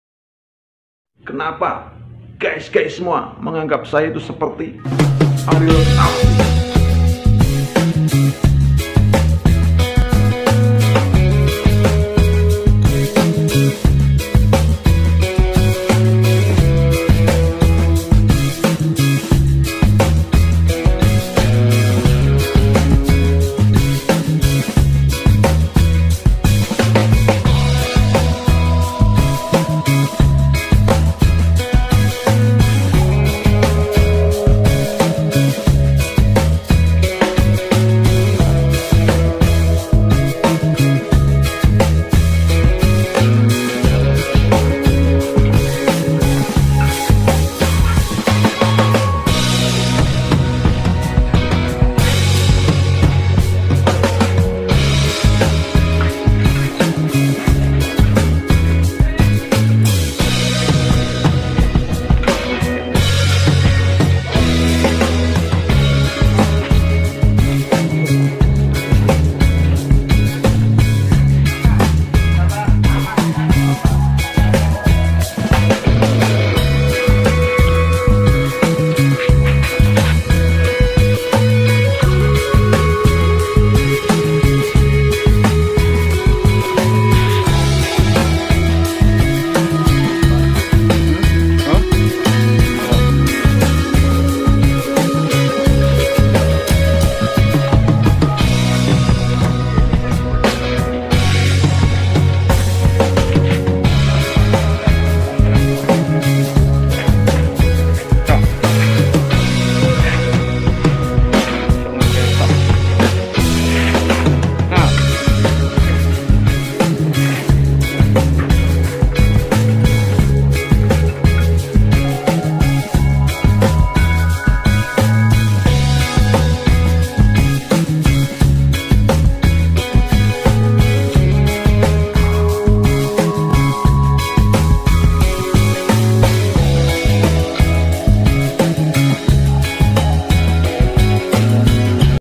Video lucu pendek Foto dan Kerjaan Ariel Noah KW
Video lucu pendek Foto dan Kerjaan Ariil Noah KW,
Di video dengan judul foto dan Kerjaan Ariel Noah KW ini kang Dino memperlihatkan seolah olah kang Dino mirip Ariel Noah padahal jauh beda yg pasti cuma lelucon kang Dino agar bisa menghibur penonton.

semoga bisa menghibur dan senyum jangan lupa panteng terus channel saya agar ngak ketinggalan video video terbaru dari kang Dino.

kerjaan Ariel Noah kw